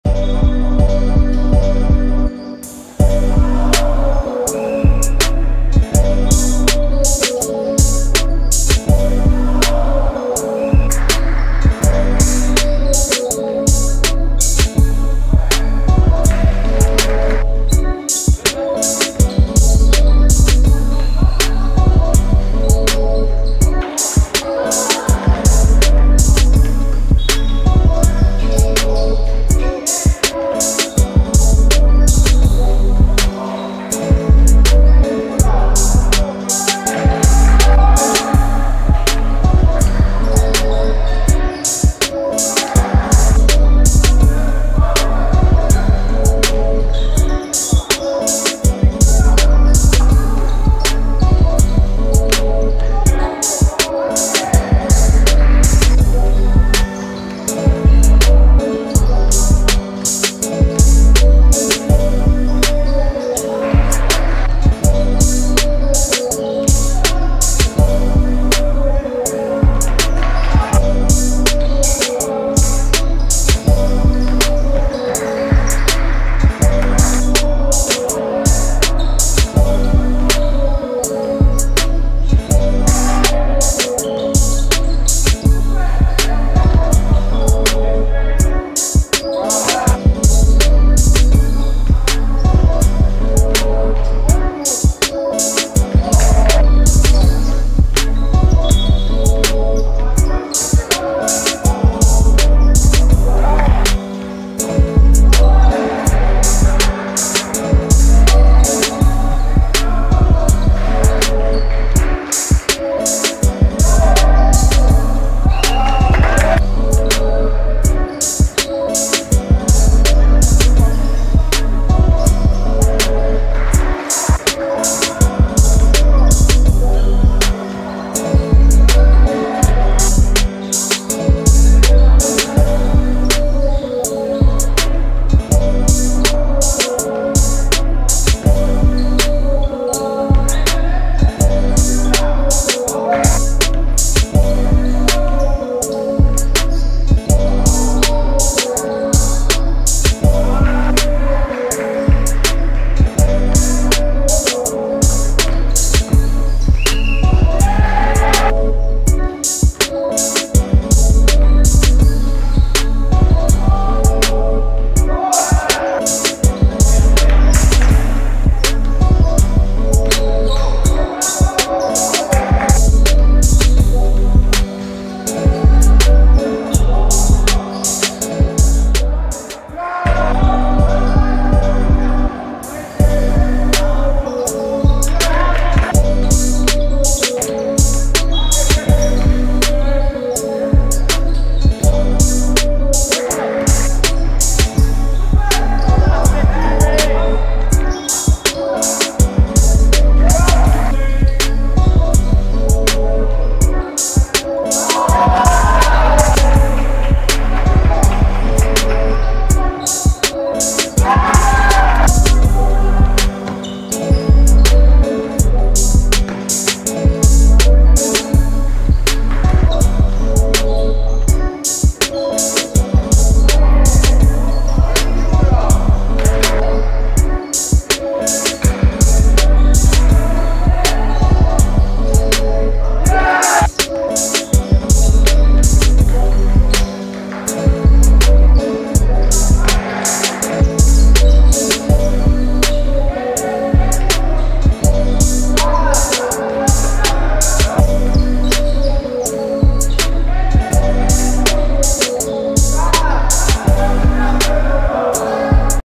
05 11 Villanova vs Ucla – Division 2 Jesień 2022
Villanova 43:50 UCLA
S.Zejer 10pkt 20zb 4ast
A.Pękacki 13pkt 6-7fg 10zb 4stl